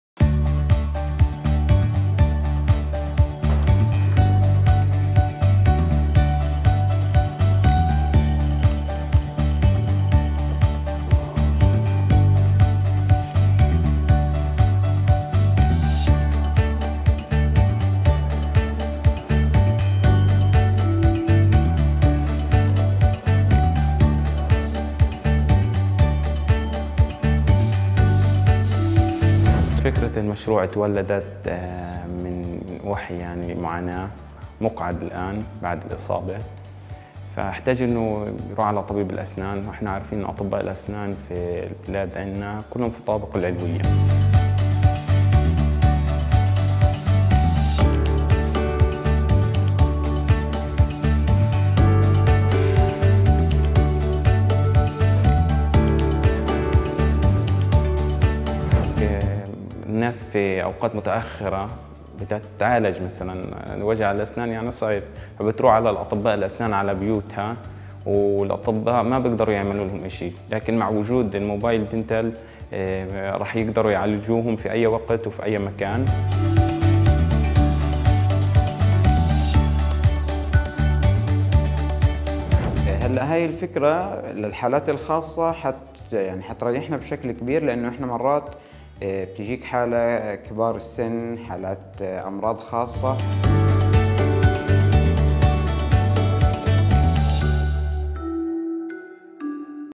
مهندس فلسطيني يبتكر عيادة أسنان بشكل حقيبة سفر
بهدف معالجة المرضى في أي وقت.. مهندس من قطاع غزة يبتكر عيادة أسنان متنقلة في شكل حقيبة سفر

ـــــــــــــــــــــــــــــــــــــــــــــــــــــــــــــــــــــــــــــــــــــــــــــــــــــــــــــــــــــــــــــــــــــ
تيلغرام
سناب شات alarabiya
تفاعلكم
صباح العربية
بانوراما
العربية الليلة
Live Alarabiya
Alarabiya Live